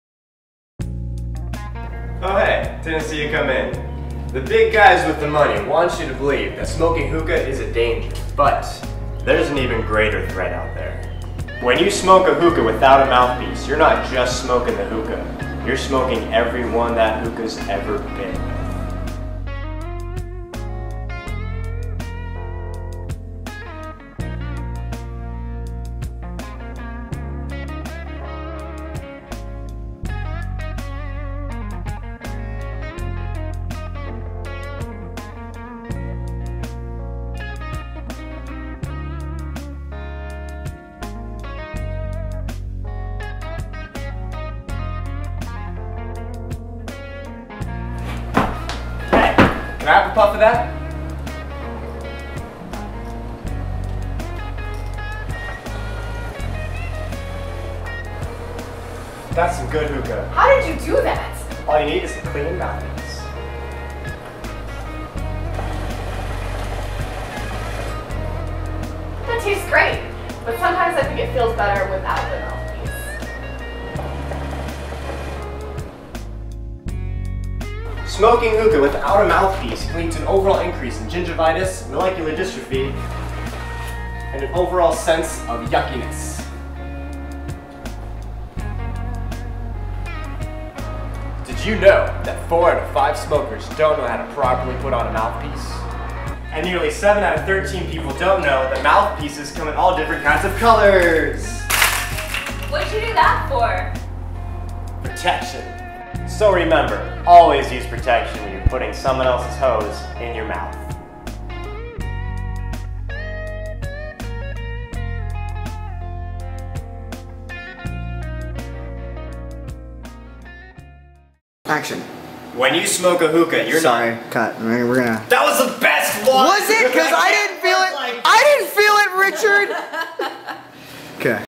Hookah PSA
The media wants you to think that smoking hookah will make you grow a second head, breathe sulfur, and vote for Trump. Here at Naked Man we know of an even worse threat!